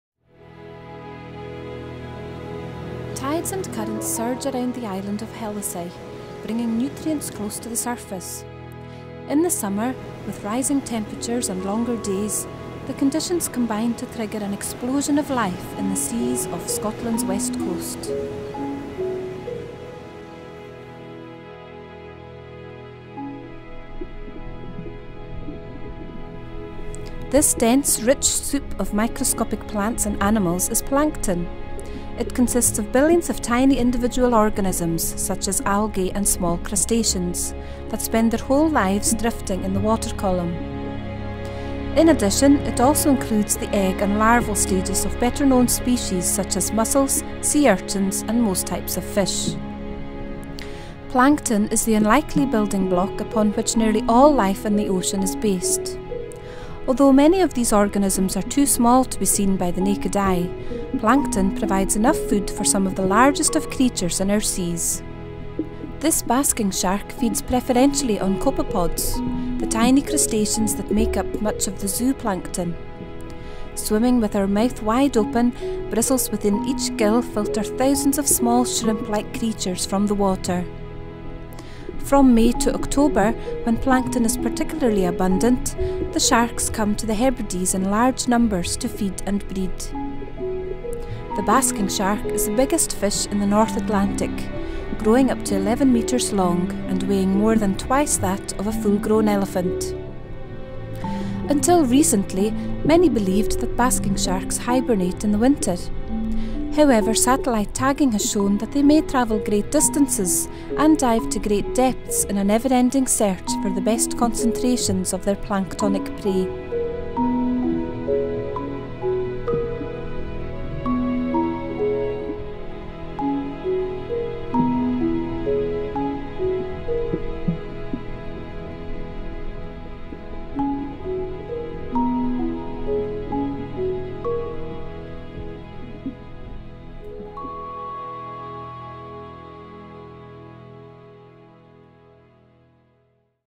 Virtual dive with basking sharks on Scotland’s west coast
This is one of eight virtual dives developed by Scottish Natural Heritage exploring Scotland’s west coast. As Scotland’s seas begin to warm after the winter, there is an explosion of microscopic life providing plentiful food for the basking shark. Each summer, high numbers of basking sharks come to Hellisay Island in the Hebrides to feed on these tiny planktonic creatures. This virtual dive provides an up close encounter with these gentle giants of the sea.